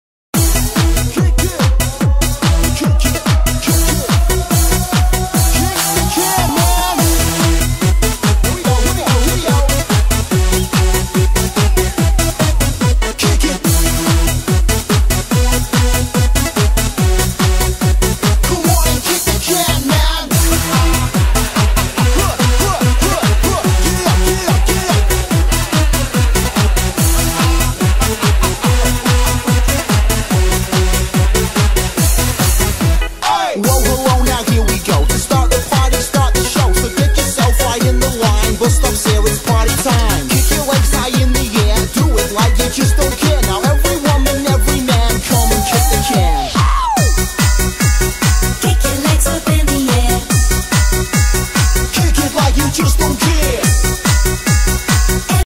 Pivot-Kick The head
a STICKMAN THAT PULLS OF PEOPLE'S HEAD OFF.to play soccer with it.
and he get kicked in the balls by everytime